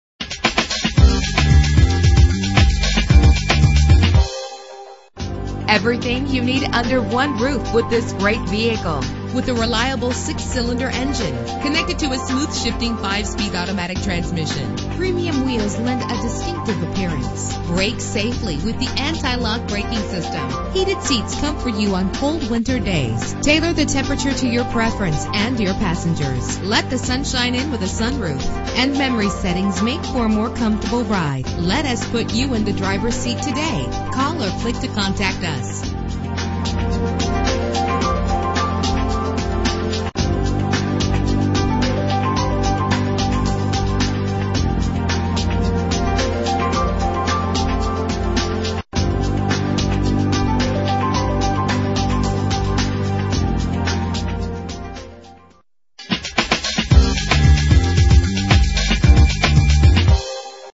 2010 Honda Accord Crosstour - Old Bridge NJ
Year: 2010
Make: Honda
Model: Accord Crosstour
Trim: EX-L
Engine: 3.5 liter 6 cylinder 24 valve
Transmission: 5-Speed Automatic
Color: Red
Mileage: 27075
Address:
1101 US highway 9